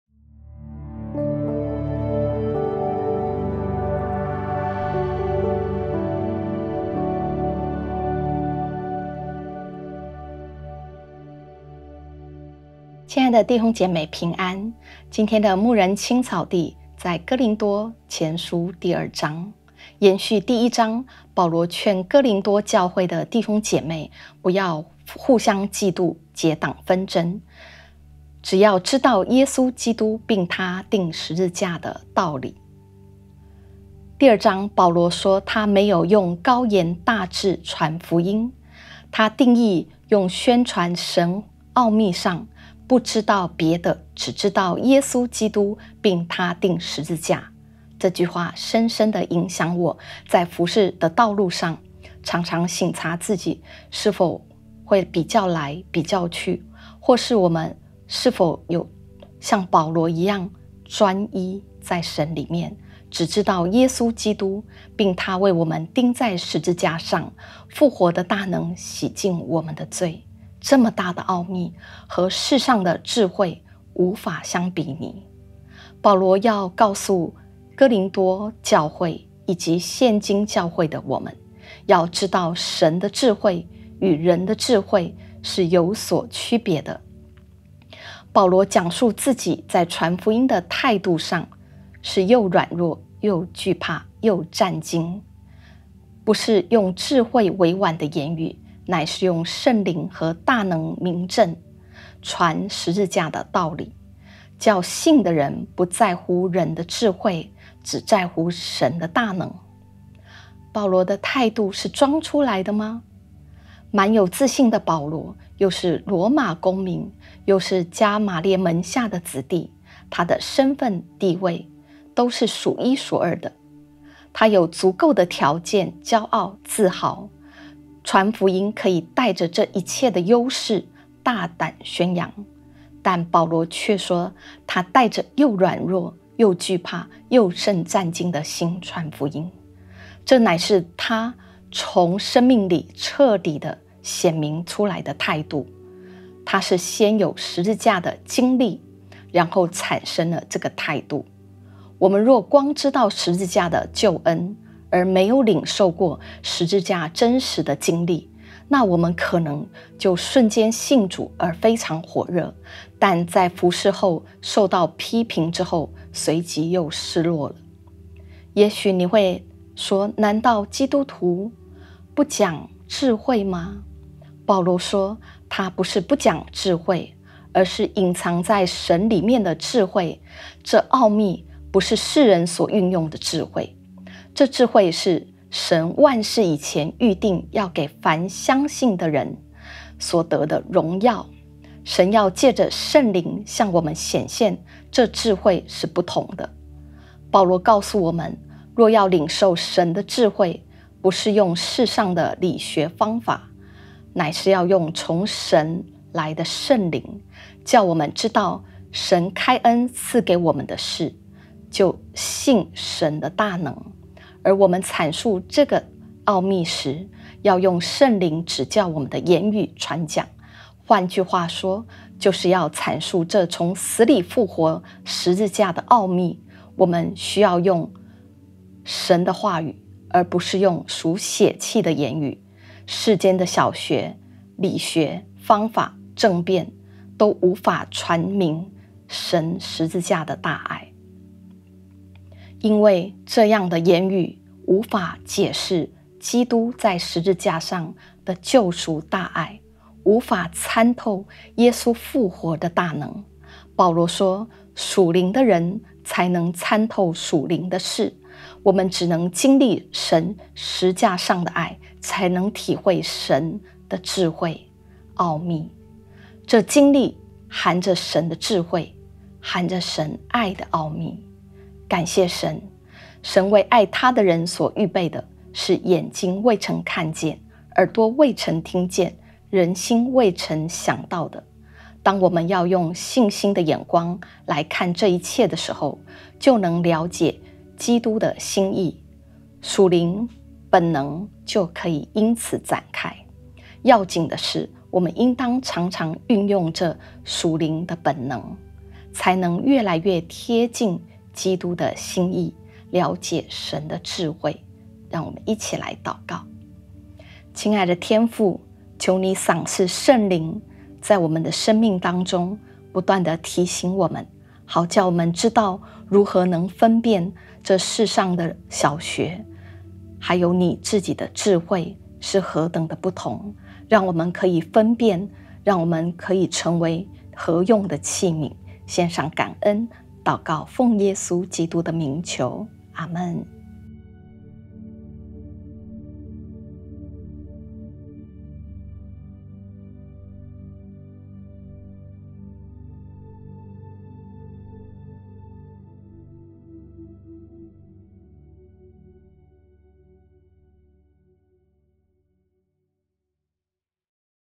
不知別的，只知耶穌│哥林多前書2│20240503牧人青草地│EP686
因為我曾定了主意，在你們中間不知道別的，只知道耶穌基督並他釘十字架。林前2:2
經文閱讀
✧ 台南市安平府前四街90號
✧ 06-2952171